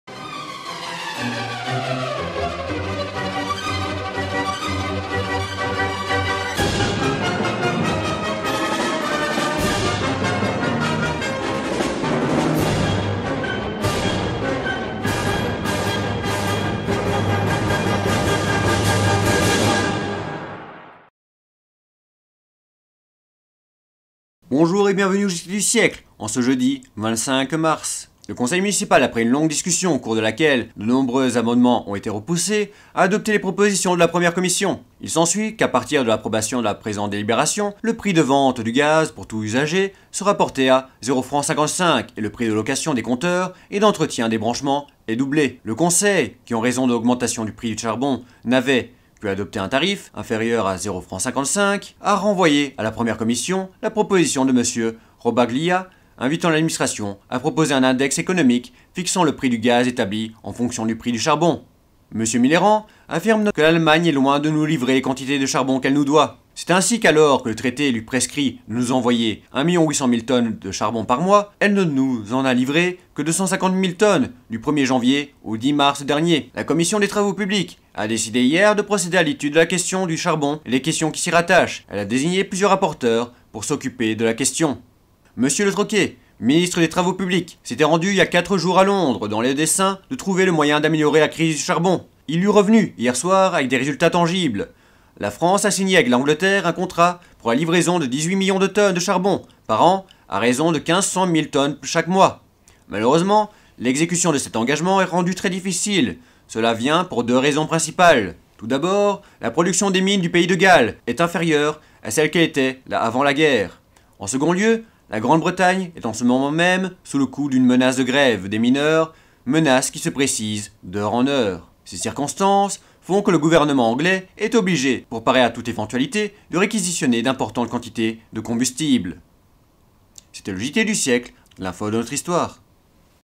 JTS 1920 03 25 Le gaz à Paris coûtera 0 fr 55 le mc, et le prix des accessoires sera doublé
L'actu la plus récente d’il y a cent ans jour pour jour
C'est le JT du Siècle, l'info de notre histoire

Jeudi 25 mars 1920

Le gaz à Paris coûtera 0 fr. 55 le mc, et le prix des accessoires sera doublé
Le Conseil municipal, après une longue discussion au cours de laquelle de nombreux amendements ont été repoussés, a adopté les propositions de la première commission.
Il s'ensuit qu'à partir de l'approbation de la présente délibération, le prix de vente du gaz pour tous usages sera porté à 0 fr. 55 et le prix de location des compteurs et d'entretien des branchements est doublé.
Le Conseil, qui en raison de l'augmentation du prix du charbon n'avait, pu adopter un tarif: inférieur à 0 fr. 55, a renvoyé à la première commission la proposition de M. Robaglia invitant l'Administration à proposer un index économique fixant le tarif du gaz établi en fonction du prix du charbon.

AURONS-NOUS ENFIN DU CHARBON?
M. Millerand affirme notamment que l'Allemagne est loin de nous livrer les quantités de charbon qu'elle nous doit. C'est ainsi qu'alors que le traité lui prescrit de nous envoyer 1,800,000 tonnes par mois, elle ne nous en a livré que 250,000 tonnes du 1" janvier au 10 mars dernier.
La Commission des travaux publics a décidé hier de procéder à l'étude de la question du charbon et des questions qui s'y rattachent. Elle a désigné plusieurs rapporteurs pour s’occuper de la question.

Londres nous aide à résoudre la crise du charbon

M. Le Trocquer, ministre des travaux publics, s'était rendu il y a quatre jours à Londres, dans le dessein de trouver le moyen d'améliorer la crise du charbon. Il eu est revenu, hier soir, avec des résultats tangibles. La France a signé avec l'Angleterre un contrat pour la livraison de dix-huit millions de tonnes de charbon par an à raison de quinze cent mille tonnes chaque mois. Malheureusement l'exécution de cet engagement est rendue très difficile, cela vient pour deux raisons principales. Tout d'abord, la production des mines du pays de Galles est inférieure à ce qu'elle était avant la guerre. En second lieu, la Grande-Bretagne est en ce moment sous le coup d'une menace de grève des mineurs, menace qui se précise d'heure en heure.
Ces circonstances font que le gouvernement anglais est obligé, pour parer à toute éventualité, de réquisitionner d'importantes quantités de combustible.